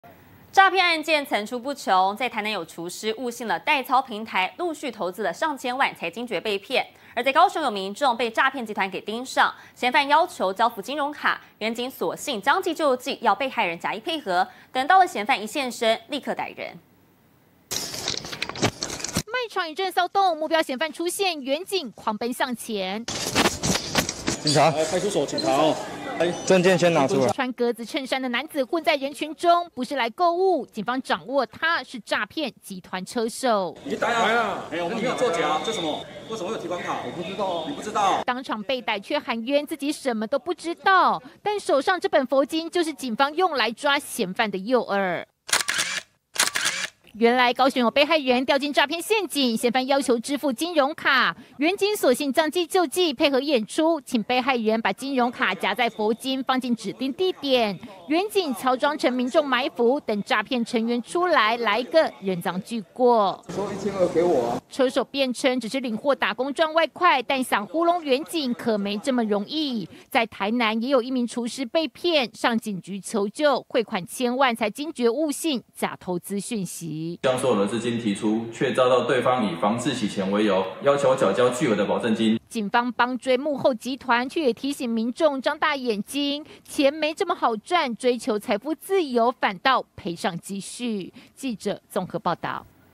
詐團要求交付金融卡 警變裝誘敵埋伏逮車手
詐騙層出不窮！台南有廚師，誤信代操平台，陸續投資千萬，後續要付高額保證金，才驚覺被騙；無獨有偶，在高雄，也有民眾被詐騙集團盯上，嫌犯窮追不捨，要求交付金融卡，員警索性將計就計，要被害人假意配合，警力喬裝民眾，佈署周邊，嫌犯出面，一擁而上逮人。